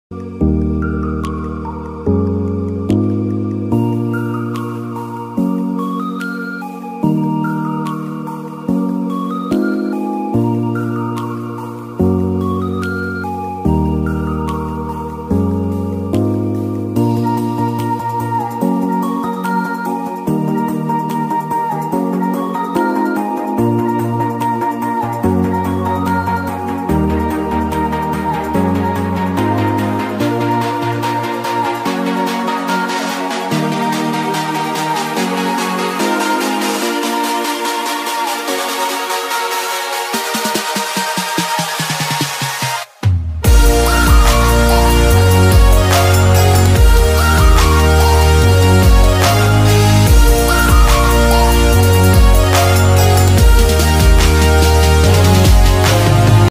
Unexpected Counter‼️|| • Pedro Damiano vs NN (1512) | Damiano Defense🔥🔥
To increase knowledge about chess.
You can visit this chess site to play and learn chess.

➣This Music Released and Provided by Fat Rat